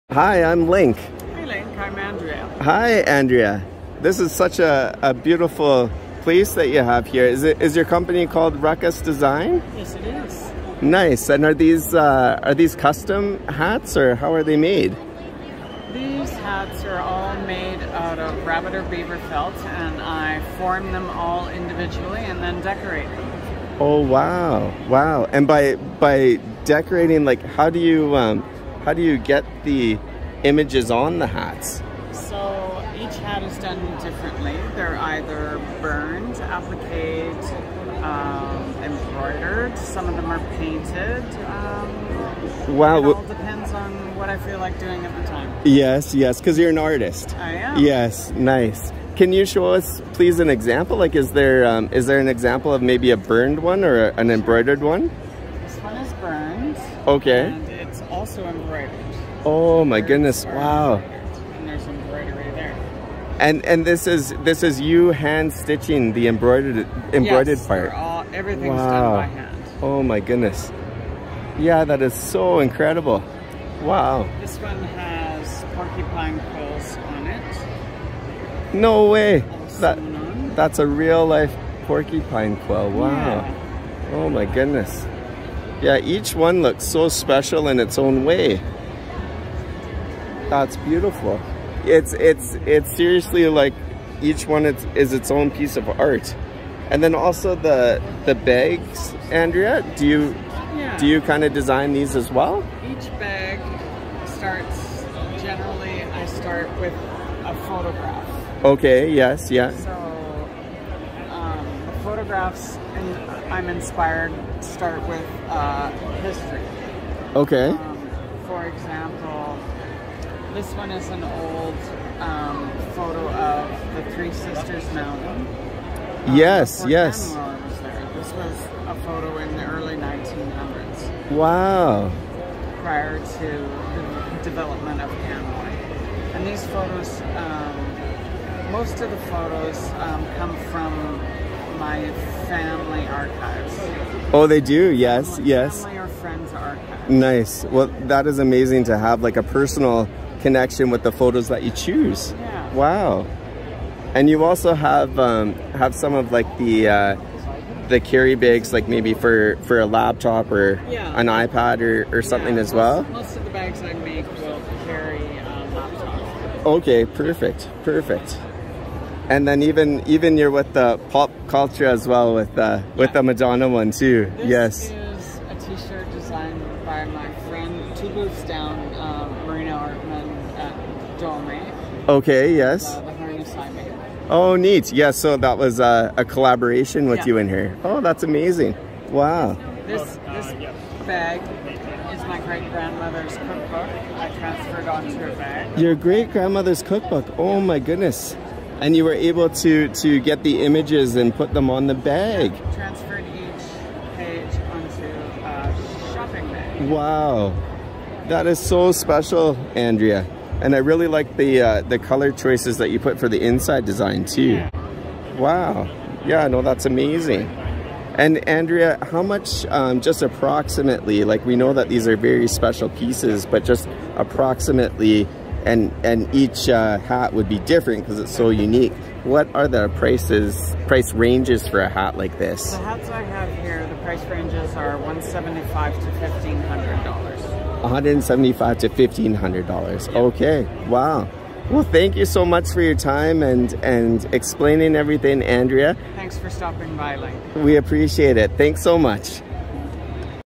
Ruckusdesign Custom Handmade Hats and more...
Andrea White is the Artist and Owner of Ruckusdesign who is very passionate about her work. Her main focus is on hats and handbags and she also has jewelry and other accessory designs. She has incorporated family pictures in her bag designs including her grandmothers cookbook being featured on one of the bags.